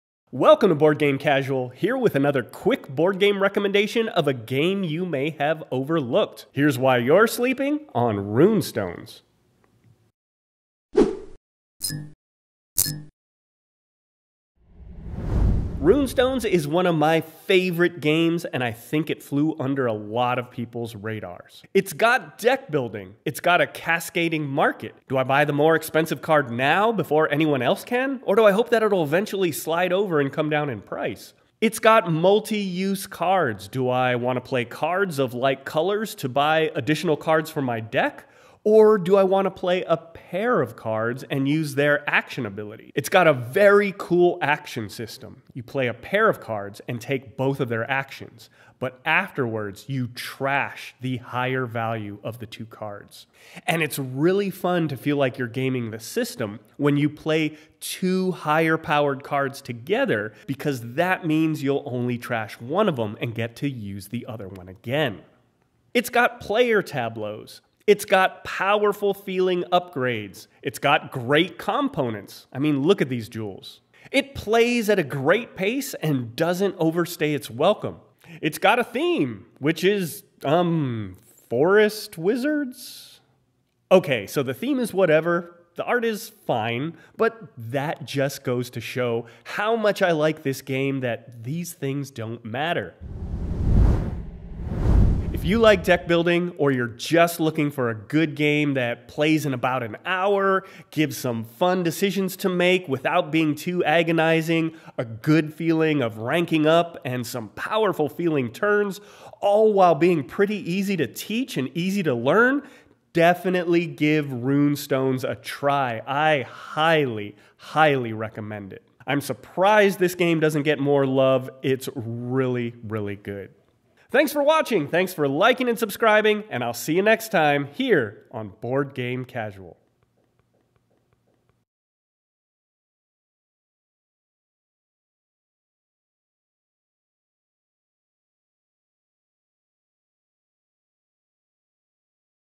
You're Sleeping on Rune Stones! (In 2 minutes)
Rune Stones is one of my favorite board games and I don't think it gets enough love!  Here's a quick video on why it's awesome, and why you should try it!

00:00 Intro
00:21 Things that make it great
01:34 Final Thoughts

You're Sleeping on Azul Summer Pavilion!

Board Game Casual:
BGG/Reddit: @Cardboard_RJ